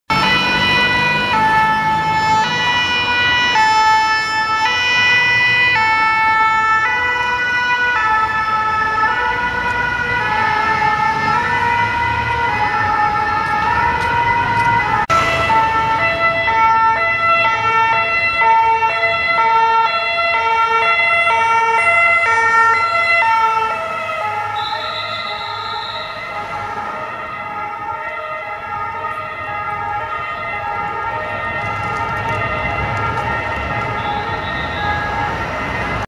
Red Cross Ambulances Responding // Croix Rouge
Two short videos of two different French Red Cross Ambulances. Notice the different sirens.